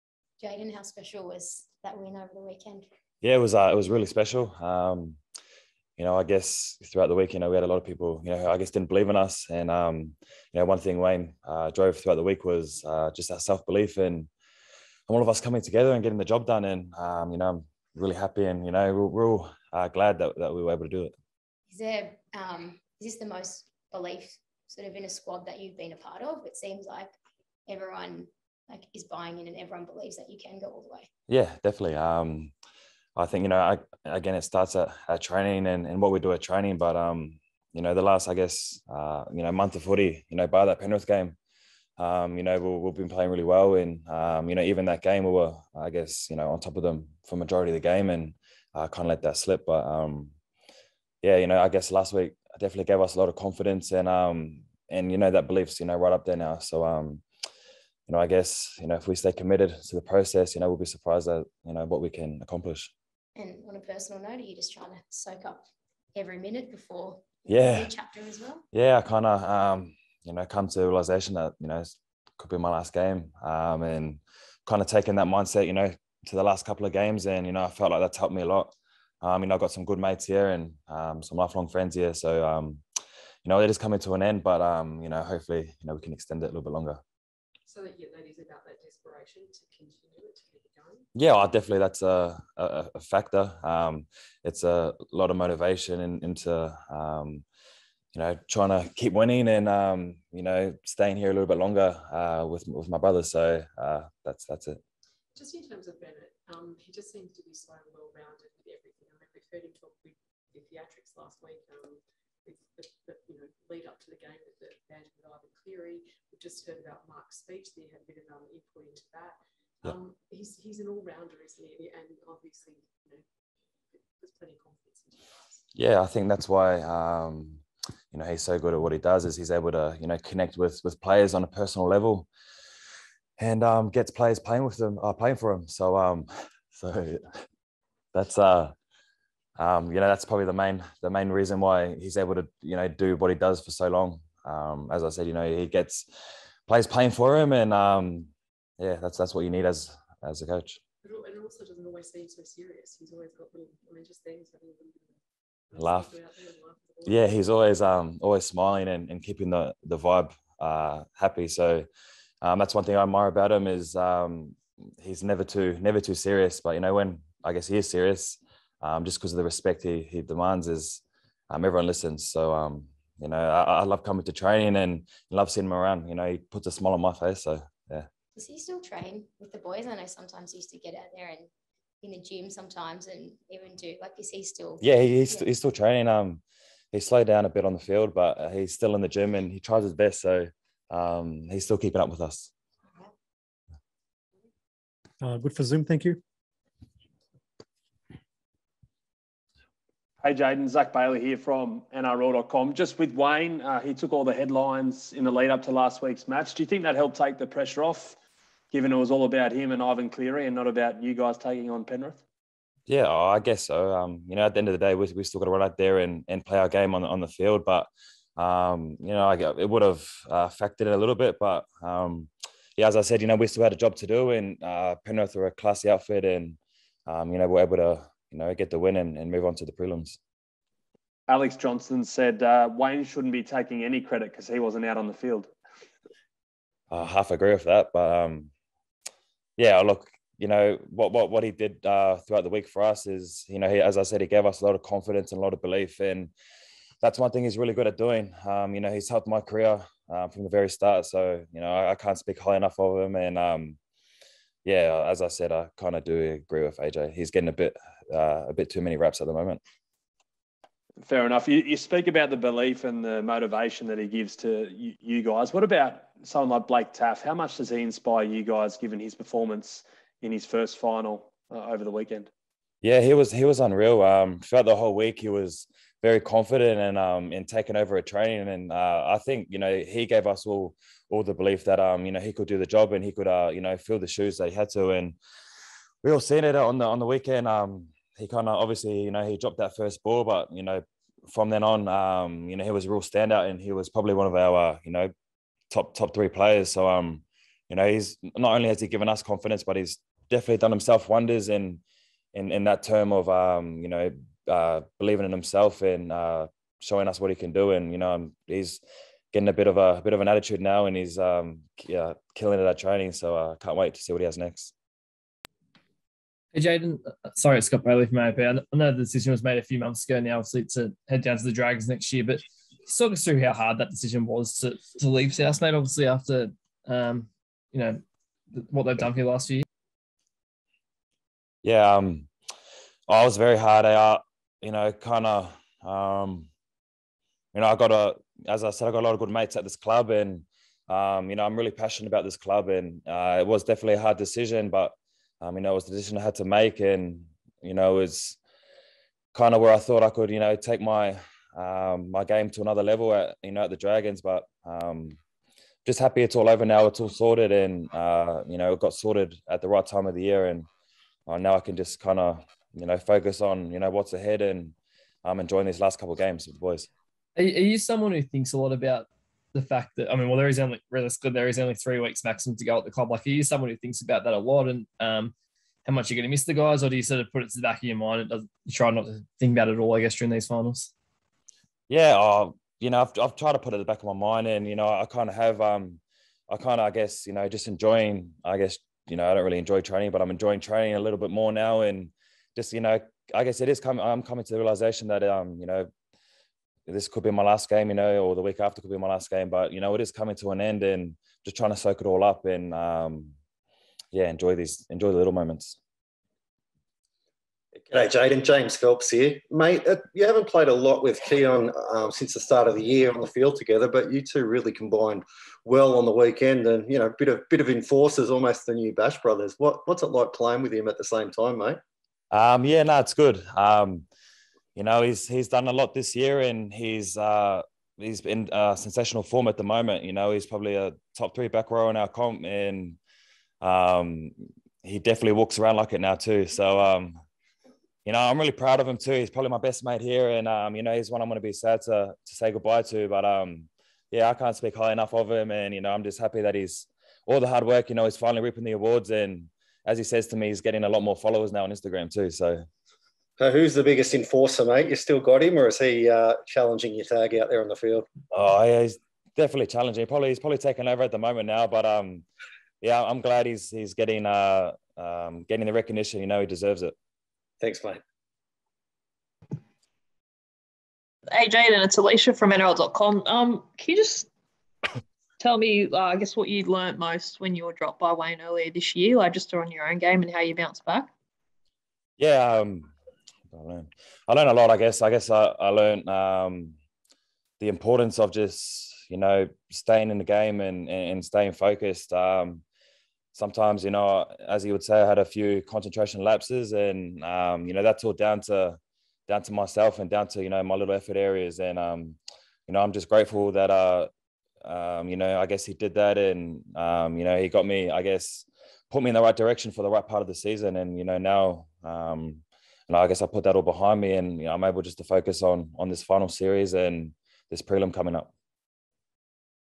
"Waynes helped my career from the very start" Jaydn Su'A
Jaydn Su'A in todays Zoom press conference.